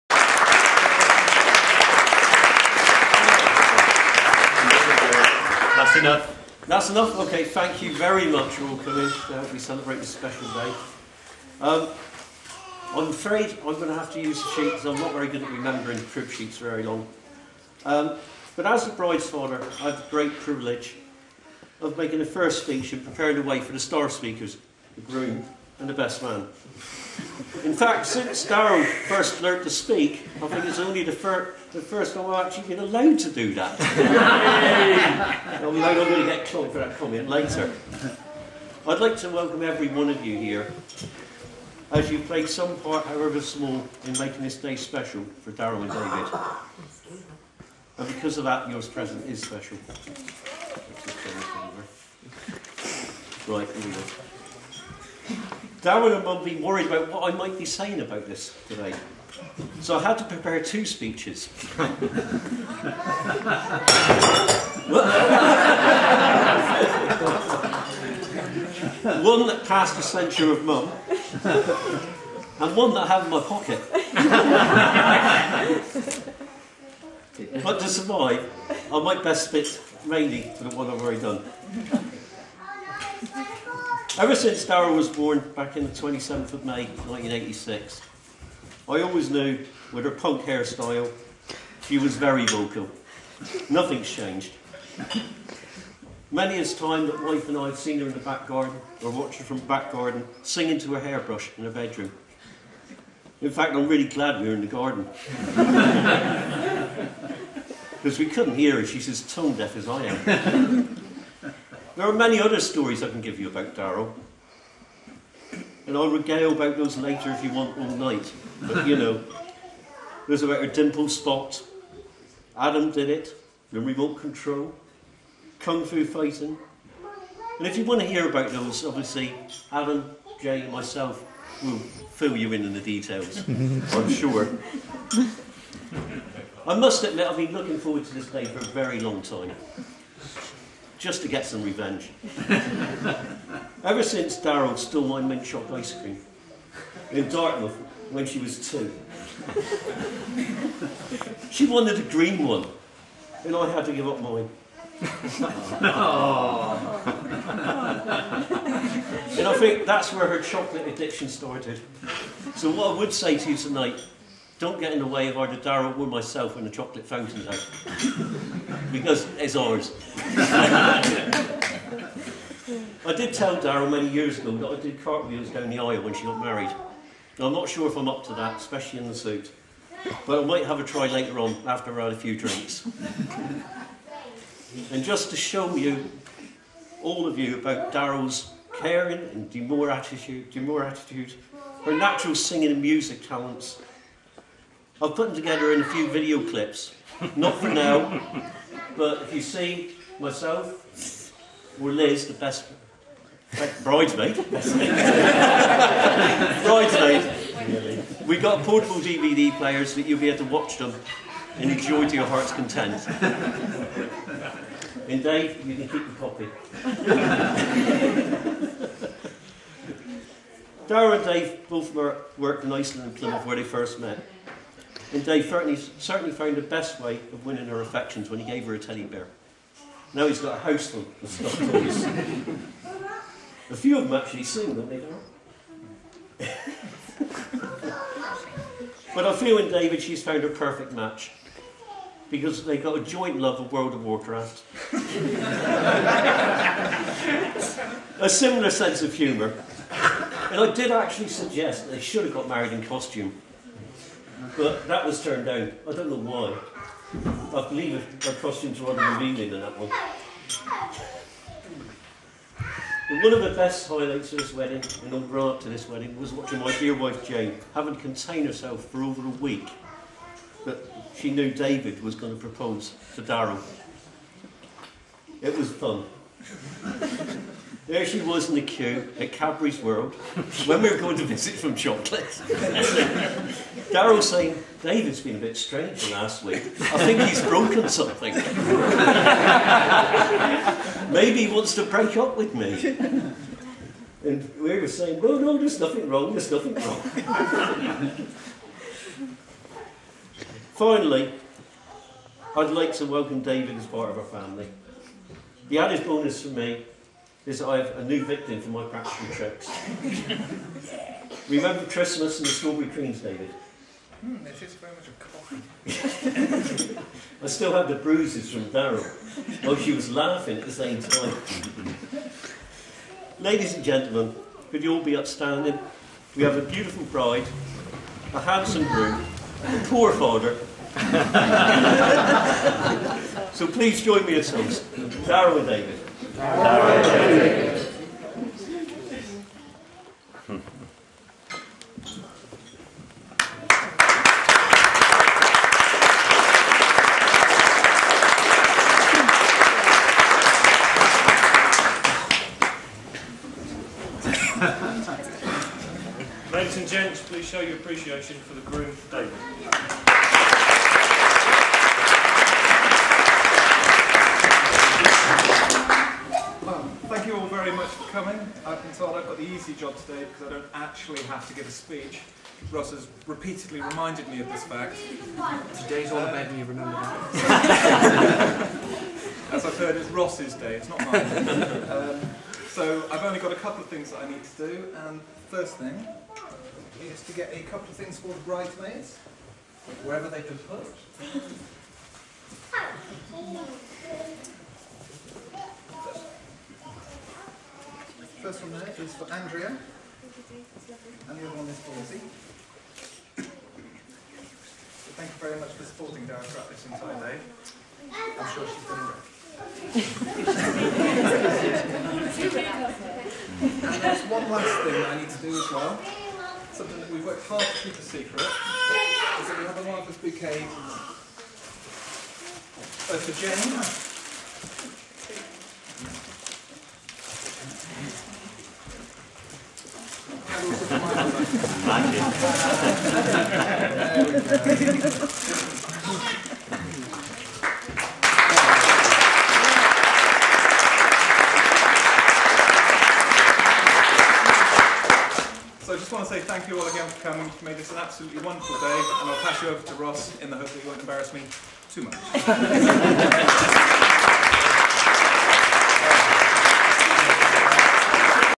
Wedding 06 - Speeches
Peter and David's speeches at the wedding